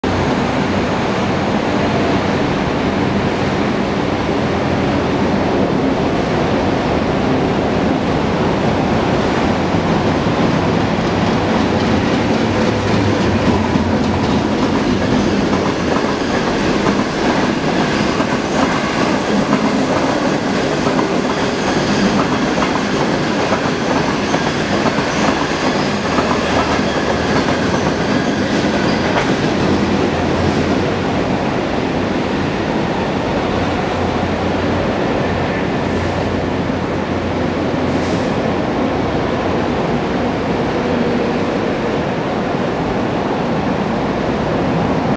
[MOR] Intercity through Train, T809, DF11 0025 Overtakes MLR. Sha Tin Station.
Intercity through Train T809 overtakes an EMU at Sha Tin Station. An old Citybus numbered 19 also captured in the video. Please note the yellow bus going away from camera at the end of the video.

Canon EOS 7D, 1920x1080 25Fps.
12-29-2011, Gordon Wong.